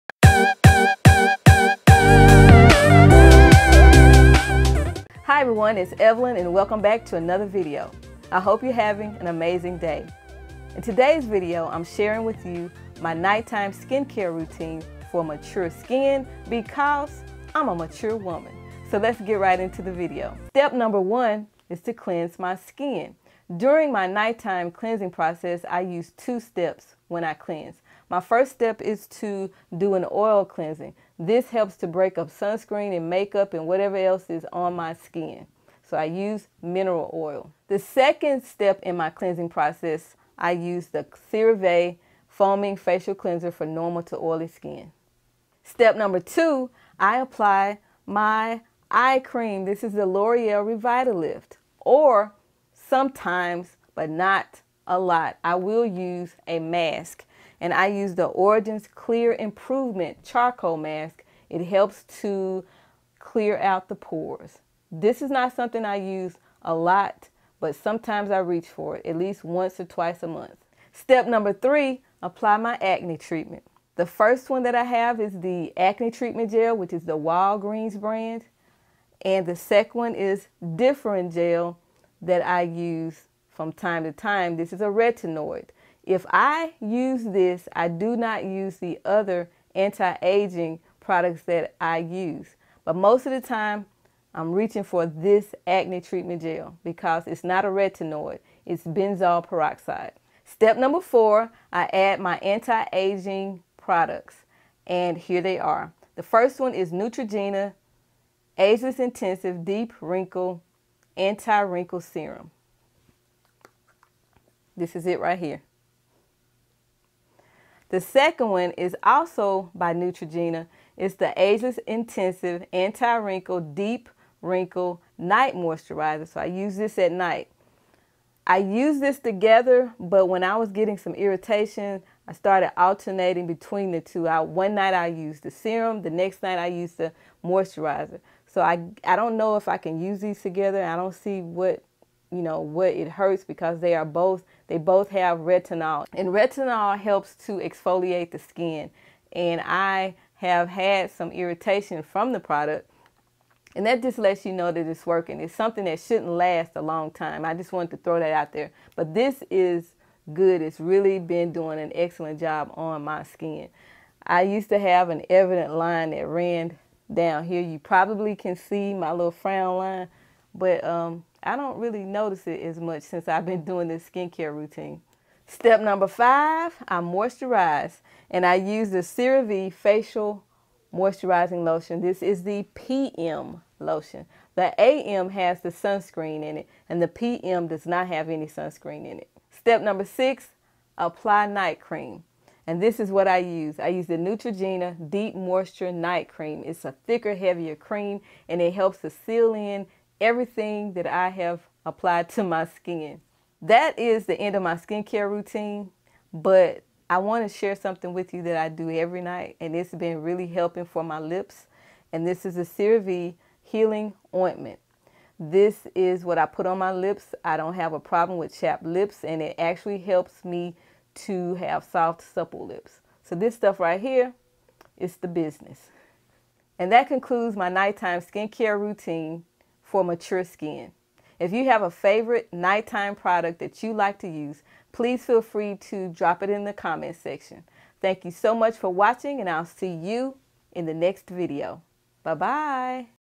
Nighttime Skin Care Routine | Women Over 40 | July 2019
Sharing my nighttime skincare routine. I've been following a consistent routine for 2-3 weeks and things are going well.

Mineral Oil

CeraVe Foaming Facial Cleanser

Loreal RevitalLife Eye Cream

Origins Clear Improvement Charcoal Mask

Differin Gel

Neutrogena Ageless Intensives Deep Wrinkle Anti-Wrinkle Serum

Neutrogena Ageless Intensives Anti-Wrinkle Deep wrinkle night moisturizer

CeraVe (PM) Facial Moisturizing Lotion

Neutrogena Deep Moisture Night Cream

CeraVe Healing Ointment